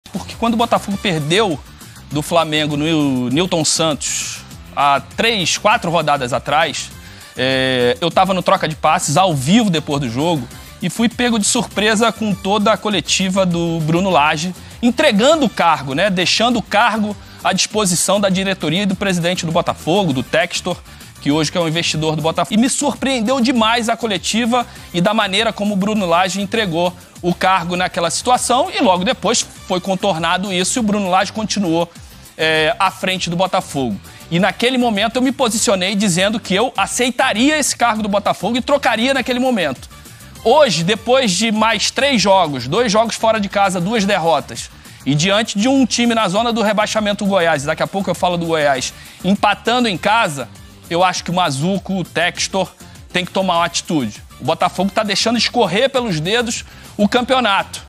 ROGER FLORES DEFENDE DEMISSÃO DE BRUNO LAGE - BOTAFOGO X GOIÁS | sportv
Roger Flores defende demissão de Bruno Lage: "Mazzuco e o Textor têm que tomar uma atitude"